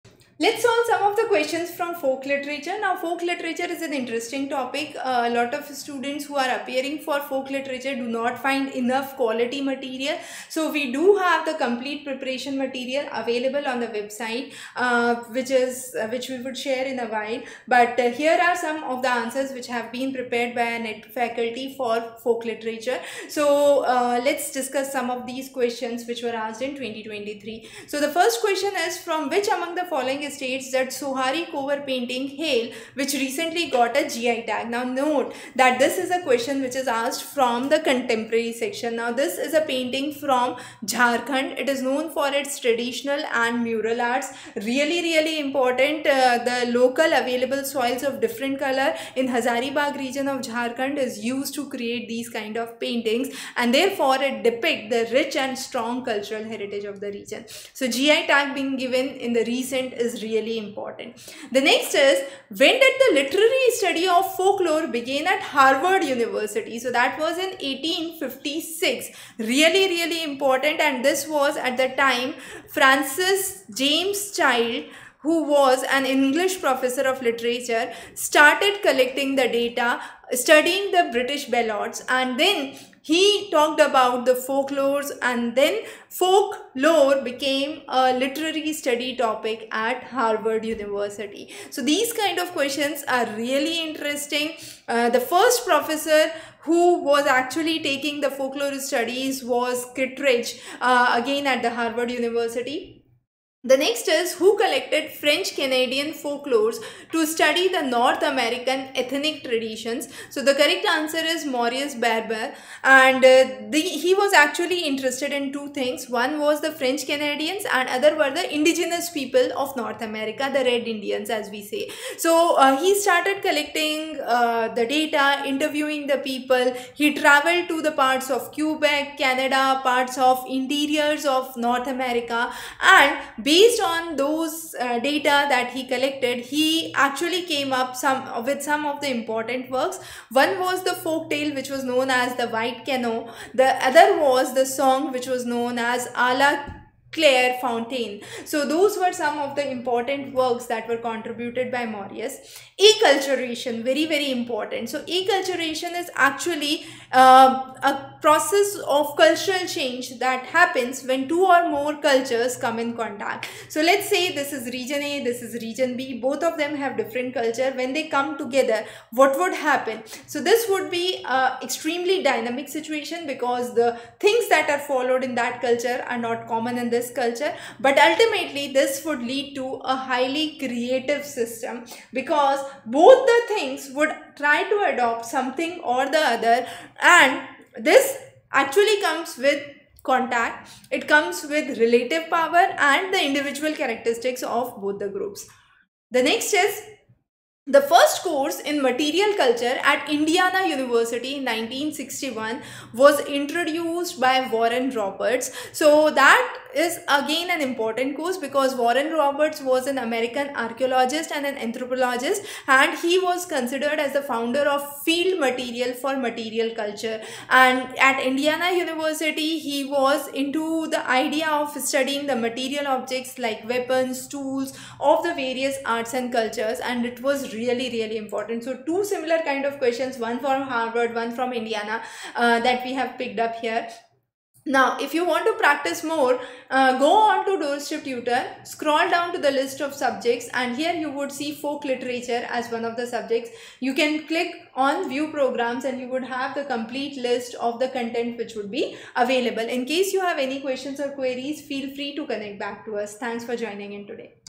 UGC NET 2023 Folk Literature Past Paper Solutions
From which among the following states does the 'Sohrai Khovar Painting' hail that recently got GI Tag?
a. Jharkhand
b. Gujarat
c. Andhra Pradesh
d. Telangana

When did the literary study of folklore begin at the Harvard University?
a. 1855
b. 1856
c. 1956
d. 1955
Who collected French-Canadian folklore to study North-American ethnic tradition?
a. Albert B. Lord
b. Ruth Benedict
c. Marius Barbeau
d. Melville J. Herskovits

. "Acculturation is not only inevitable and extremely dynamic but also highly__________"
a. Creative
b. Optimistic
c. Pessimistic
d. Conservative
The first course in Material Culture at Indiana University in 1961 was introduced by:
a. Stith Thompson
b. Warren E. Roberts
c. John Edwards
d. Archie Green